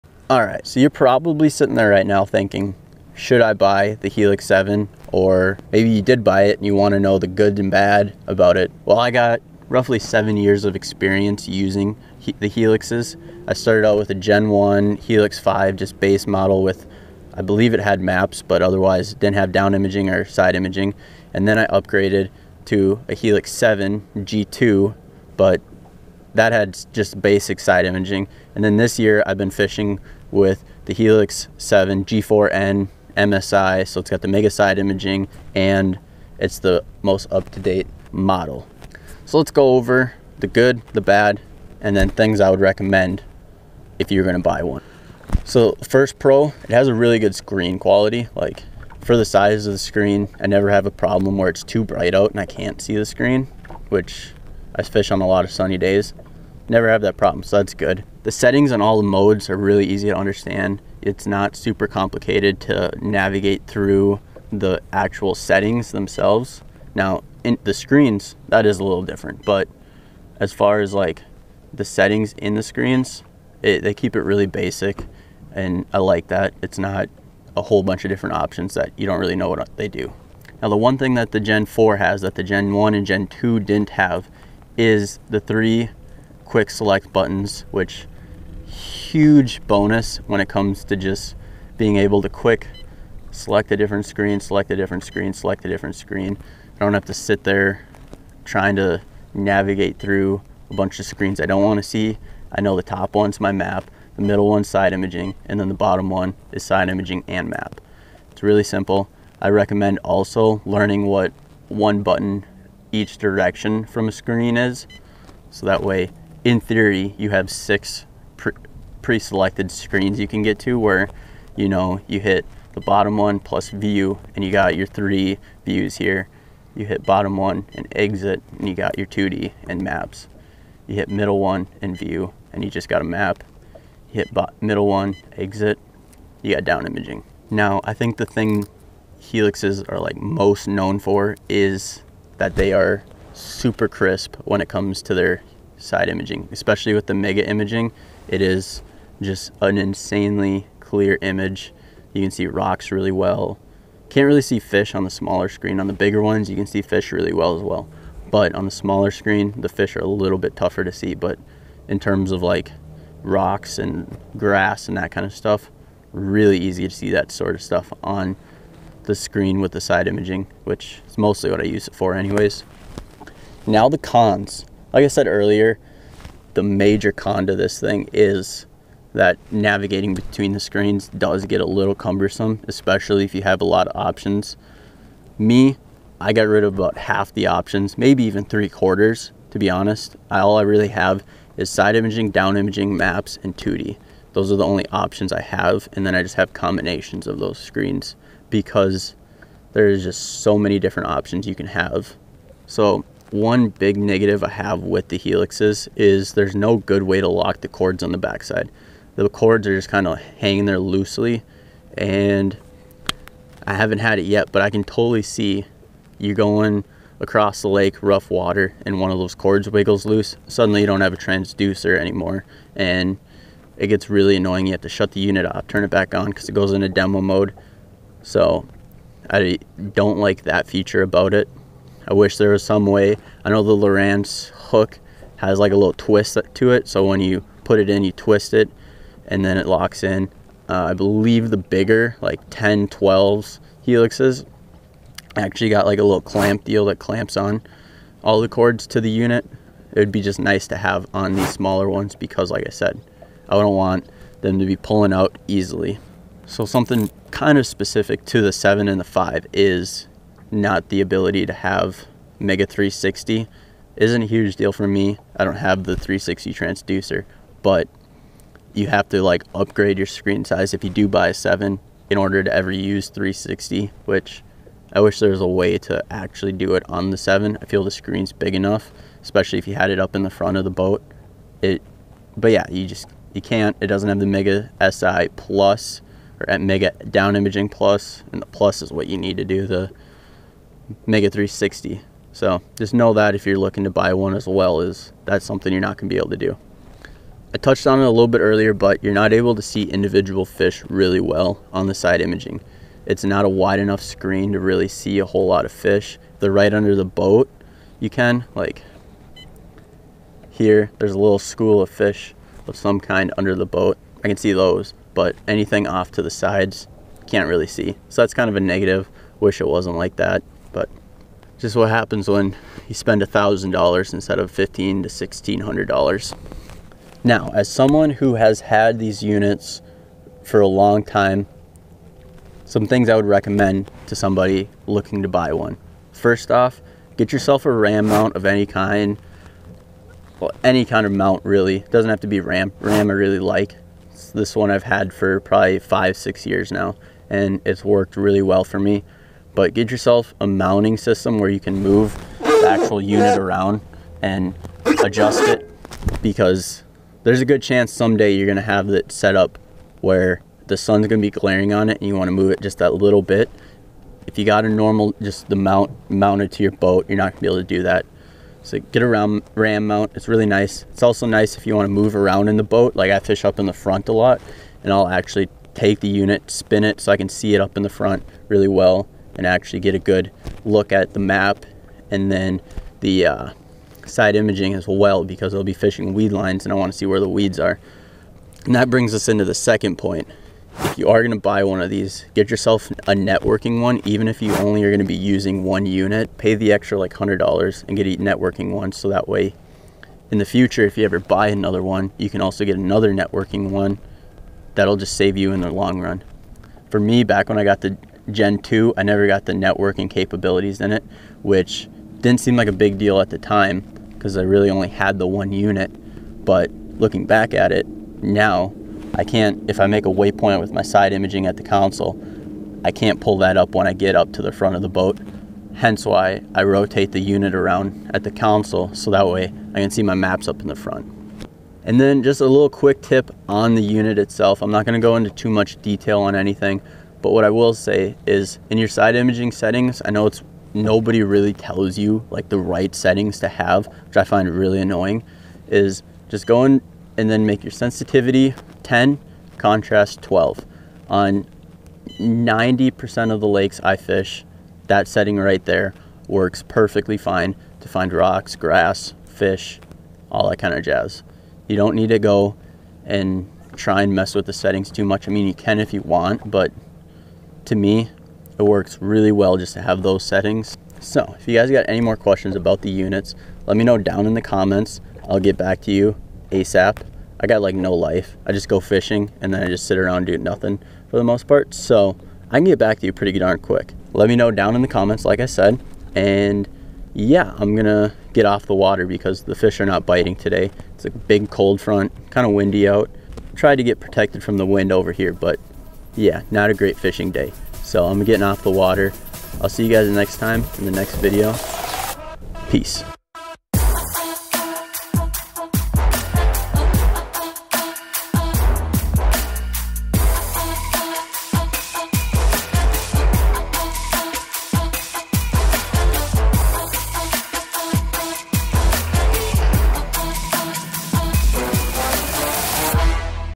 HONEST HUMMINBIRD HELIX 7 MEGA SI GPS G4N REVIEW
In todays video I give my honest review on the Humminbird Helix 7 MSI G4N after using it for the past fishing season and compare it to the past helix units I've owned (helix 5 G1 gps and helix 7 G2 SI) to give you the best idea of what you will be buying when purchasing this unit.

All in all I have good things to say and would recommend this to anyone, but with any good review video I had to say my negatives as well. I am not here to tell you to buy, or not to buy. I simply stated all the facts I know about the unit and hope you can use this information to help you in your fish finder search.